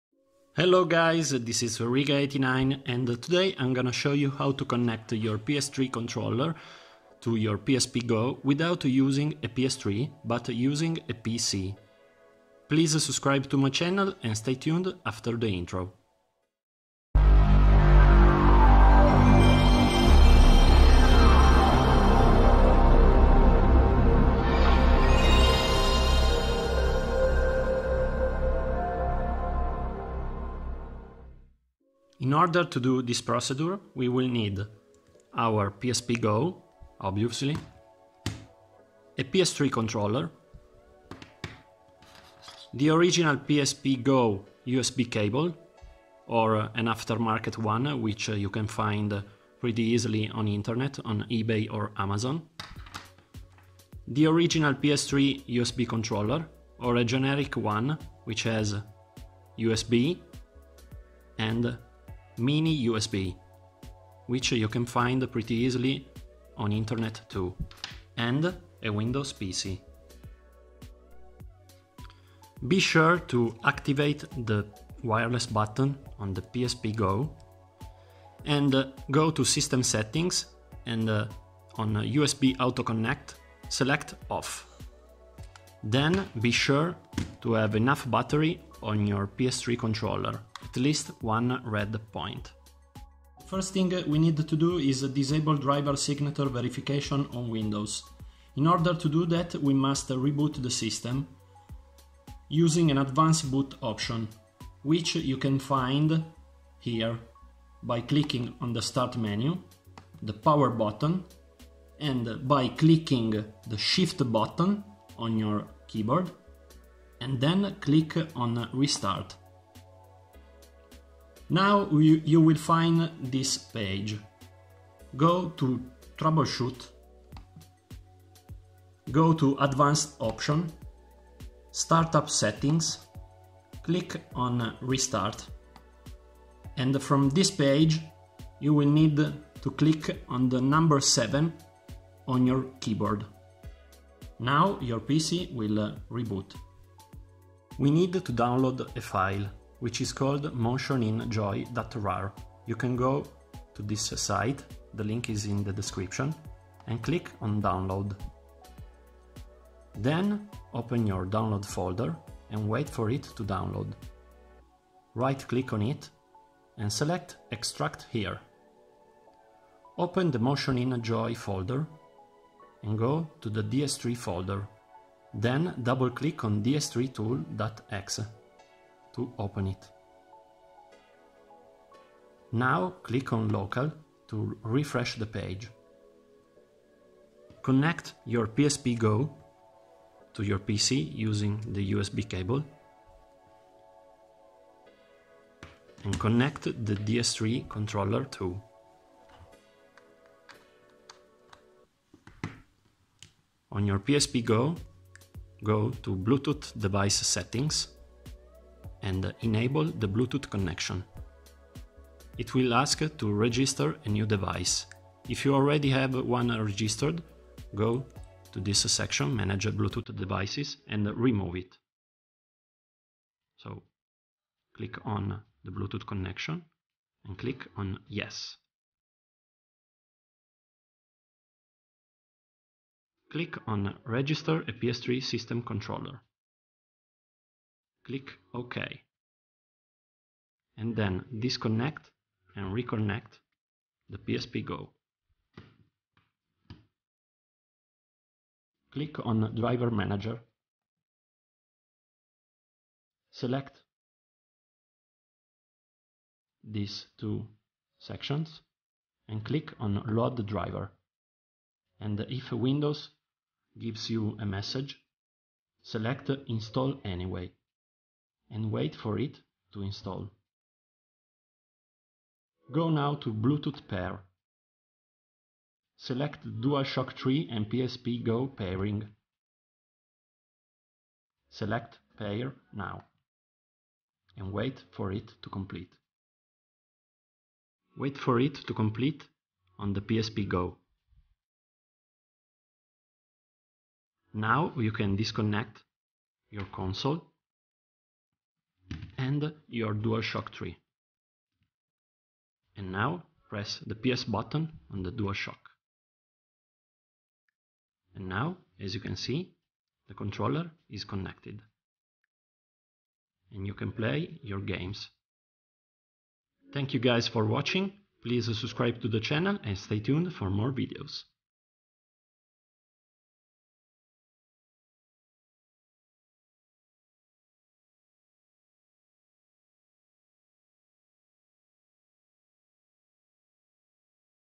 How to Connect Original DUALSHOCK 3 Controller to PSP Go WITHOUT a PS3 (Easy Method with PC)
Hello guys, this is Riga89 and in today's video I'm gonna show you how to connect a Dualshock 3 Controller to your PSP Go without using a PS3!

This method is super easy and works perfectly!